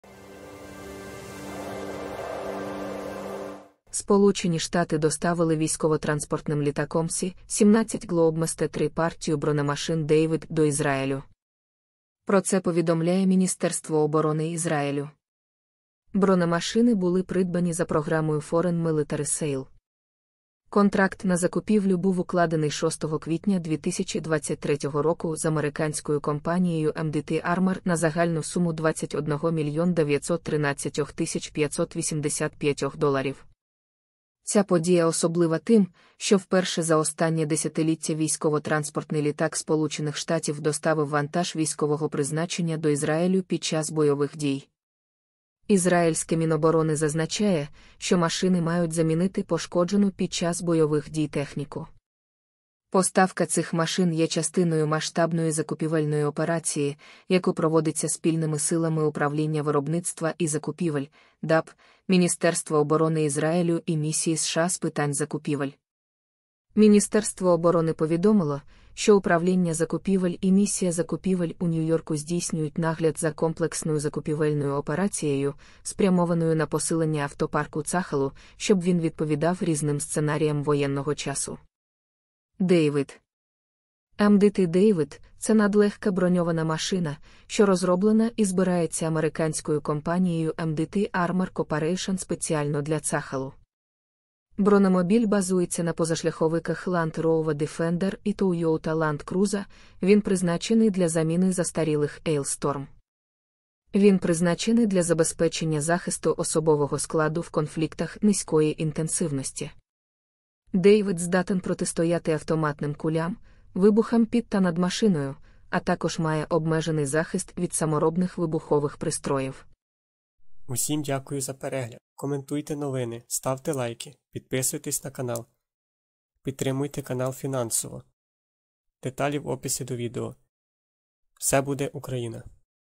Сполучені Штати доставили у Ізраїль партію бронемашин David
Знищений MDT David. Фото з відкритих джерел.